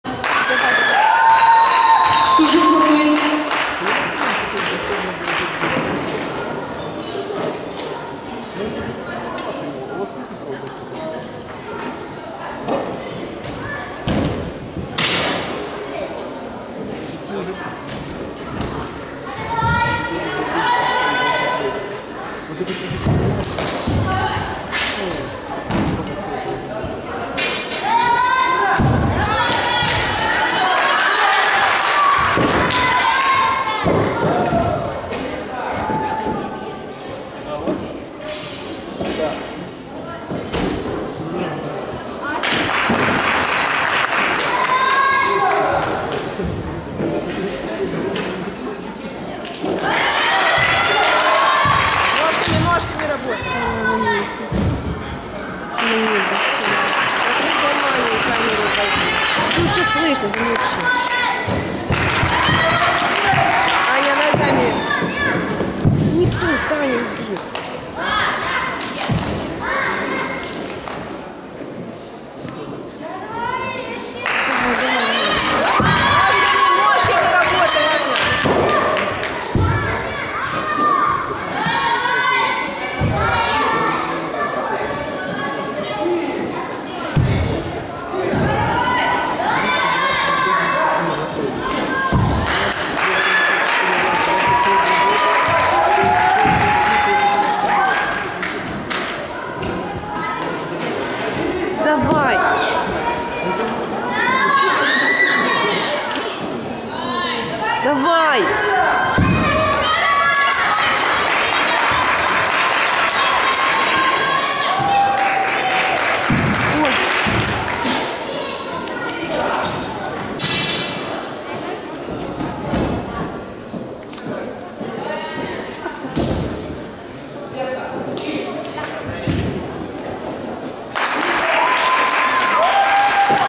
Мандрик Анна чанцюань гуйдин
Этап Кубка Украины по ушу. Дрогобыч, Львовская область. 4-6 февраля 2011.
Wushu Cup of Ukraine 2011

Одесская областная Федерация Ушу. Украина.
Выступление по ушу детей из Одессы.
The Wushu Federation of Odessa Region